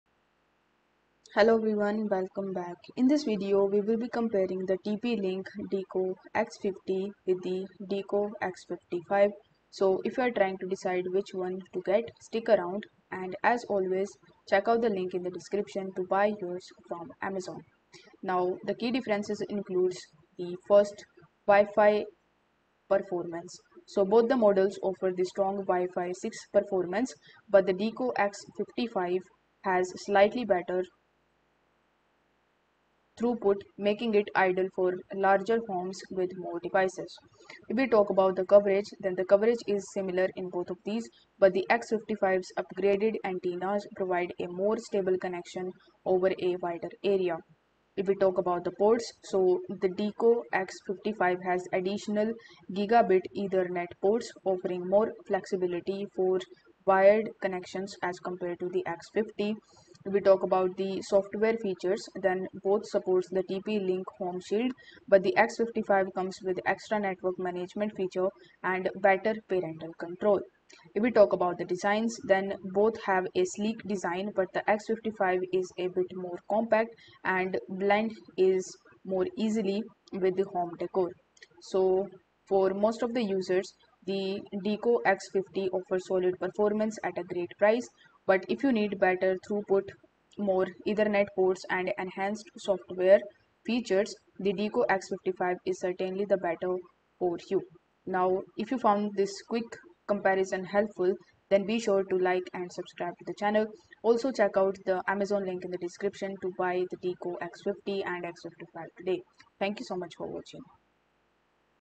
Tp link Deco x50 vs Deco x55 - Which One Is Better?
Welcome to Kwik Tuts! In this tutorial video, I will show you Tp link Deco x50 vs Deco x55 - Which One Is Better?